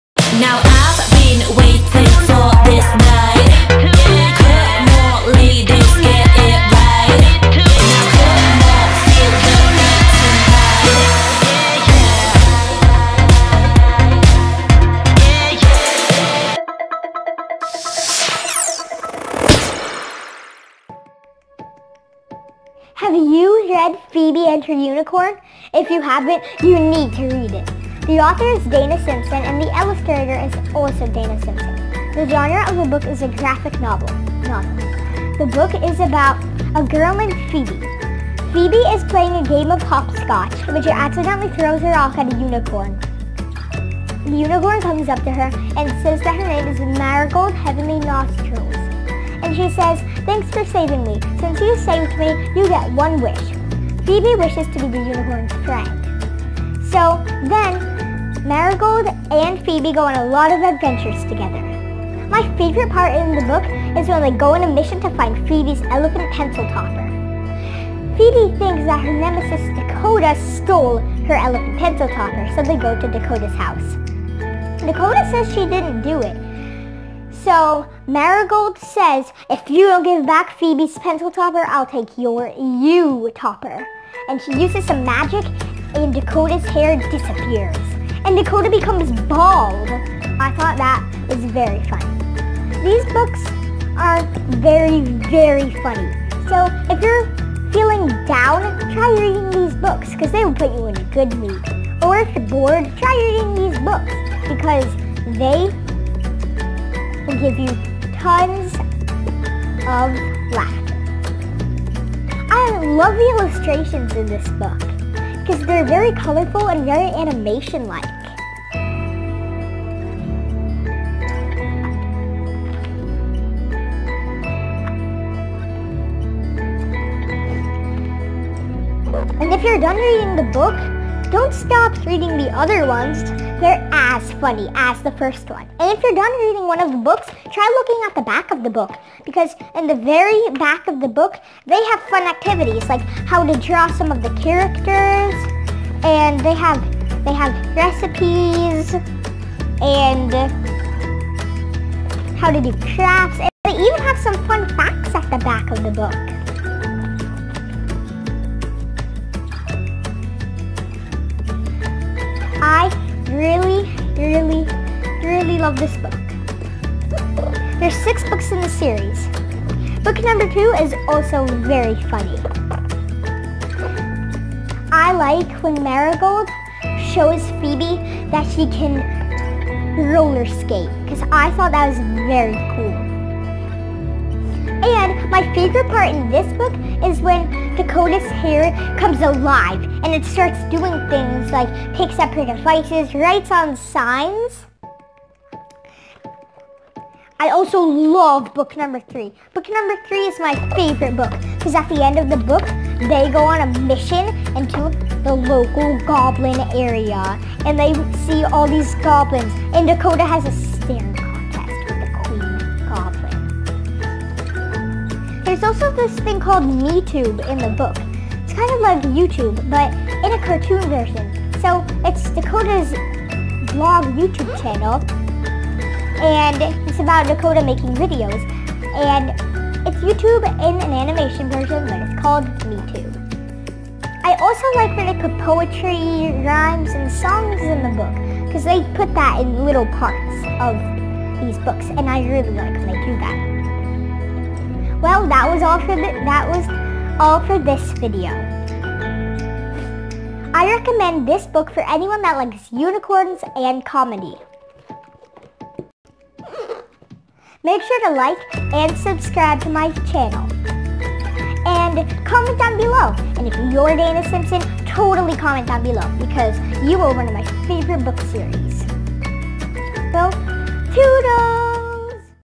Book Review Of Phoebe and Her Unicorn
Kids books reviewed by a kid! In this episode, Morgan reviews the 1st three graphic novels in Dana Simpson's unicorn series. A fun read for anyone who loves unicorns and comedy!